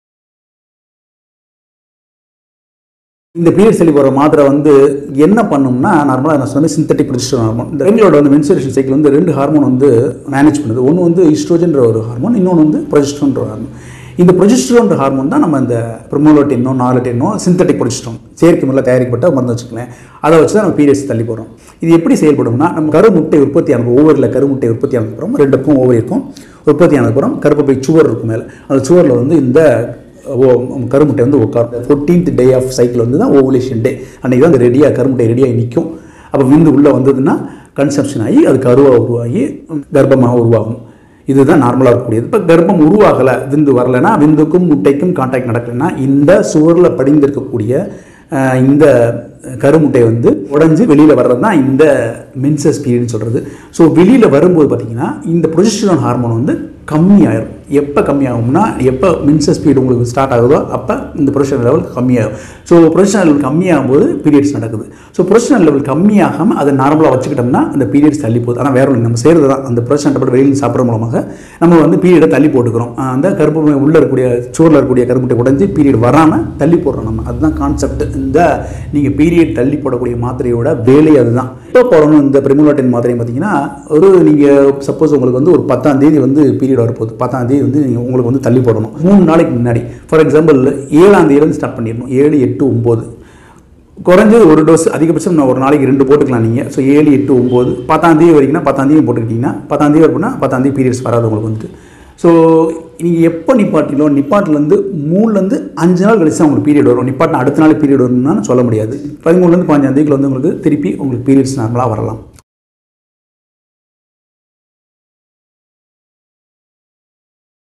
மாதவிடாயைத் தள்ளிப்போட மாத்திரை Primolut N # Norlut N
Discussed about the medicine/tablet used to postpone menses' periods. How it works, how to take period postpone tablet, dose, side effects. # Primolut N # Period postpone tablet # side effects of Primolut N # Norlut N Regestrone how it postpones periods